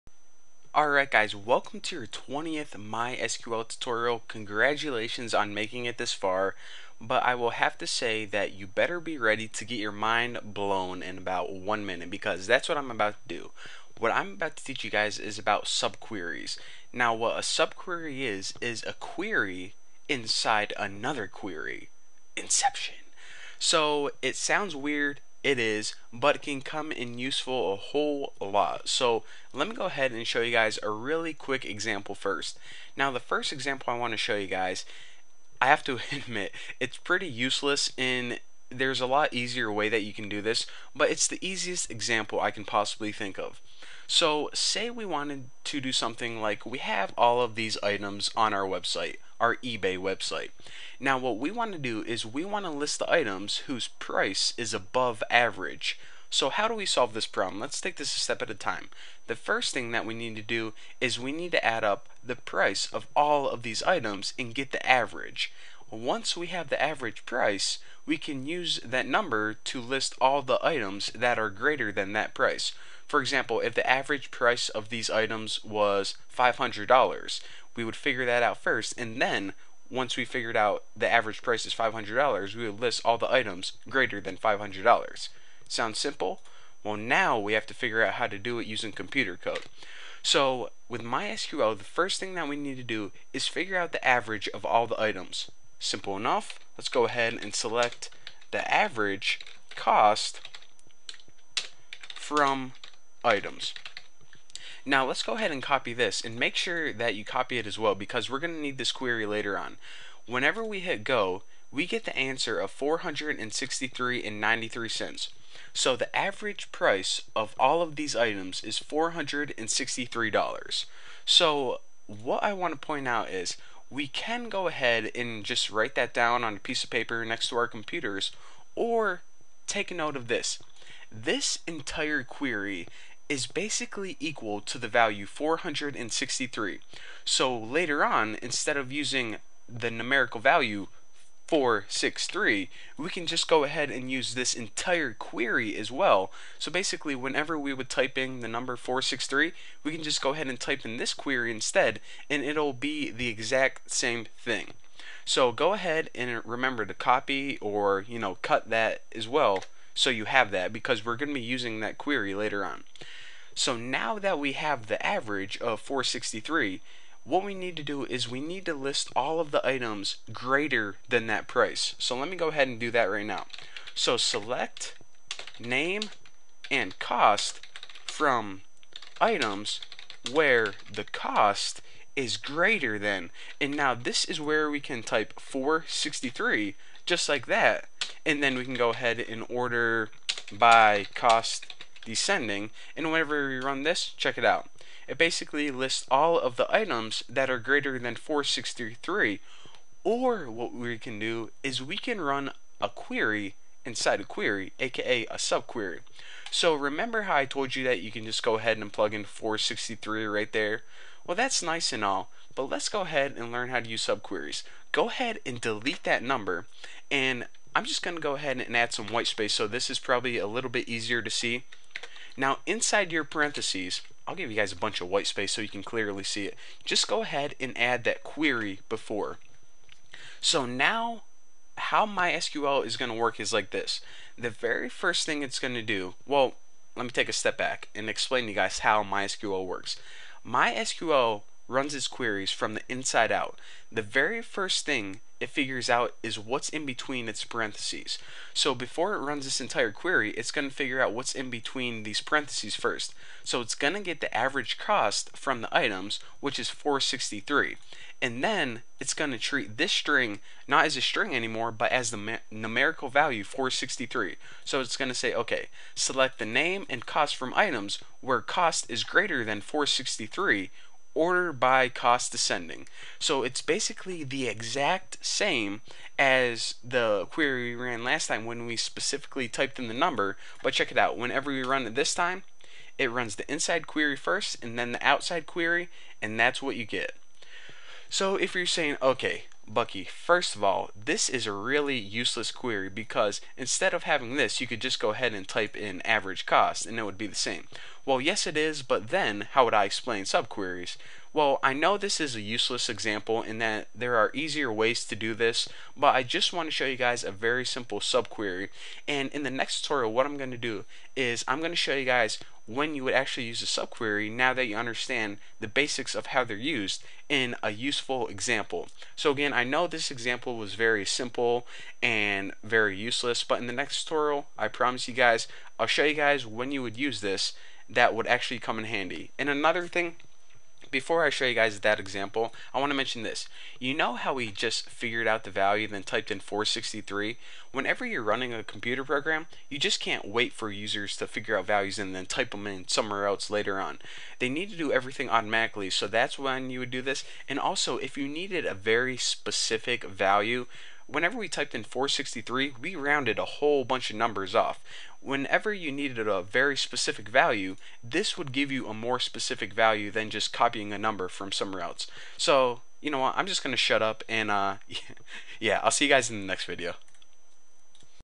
SQL Subqueries | Subqueries in SQL with examples
SQL Subqueries | Subqueries in SQL - This SQL video teaches Subqueries using northwind database example table.

SQL is a standard language for storing, manipulating and retrieving data in databases. Our SQL tutorial will teach you how to use SQL in: MySQL, SQL Server, MS Access, Oracle, Sybase, Informix, Postgres, and other database systems.